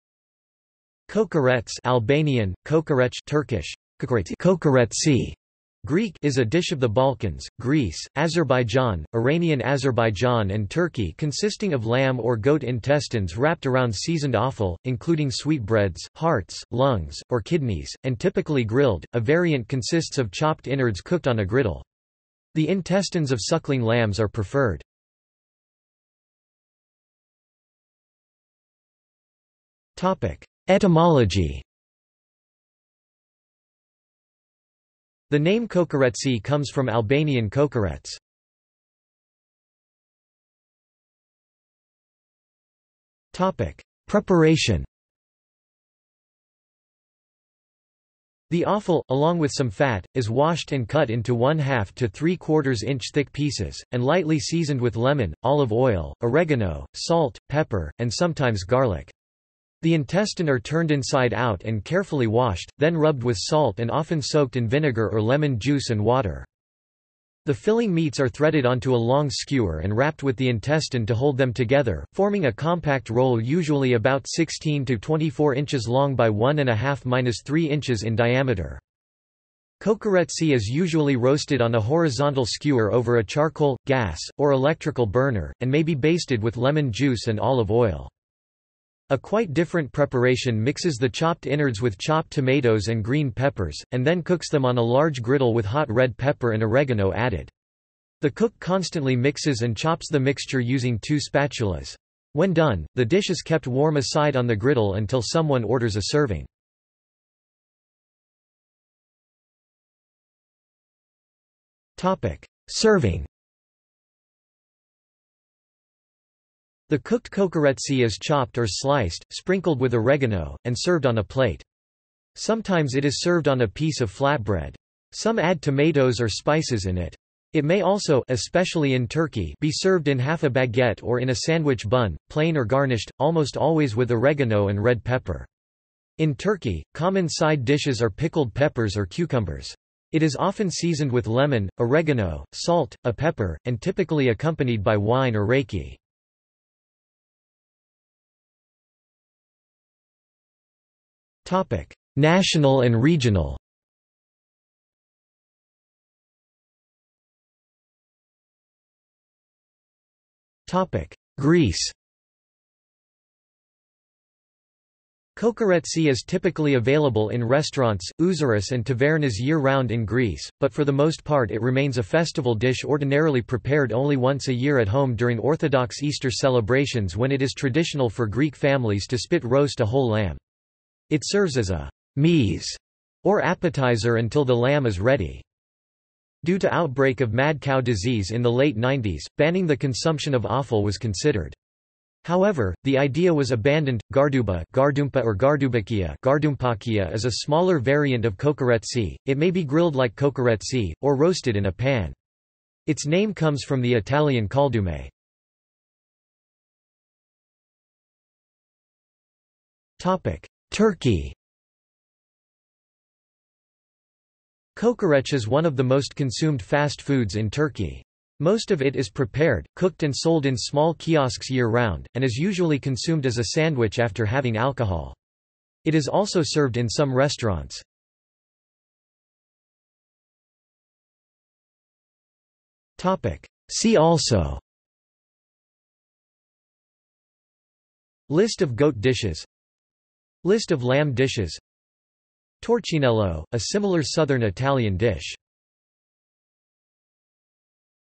Kokoretsi | Wikipedia audio article
This is an audio version of the Wikipedia Article:
Kokoretsi

00:00:26 1 Etymology
00:00:39 2 Preparation
00:01:48 3 Serving
00:02:27 4 National and regional
00:02:37 4.1 Greece
00:03:26 4.2 Turkey
00:03:49 5 See also

- Socrates

SUMMARY
Kokorets (Albanian), Kokoreç (Turkish), Κοκορέτσι "Kokoretsi" (Greek) is a dish of the Balkans, Greece, Azerbaijan, Iranian Azerbaijan and Turkey consisting  of lamb or goat intestines wrapped around seasoned offal, including sweetbreads, hearts, lungs, or kidneys, and typically grilled; a variant consists of chopped innards cooked on a griddle. The intestines of suckling lambs are preferred.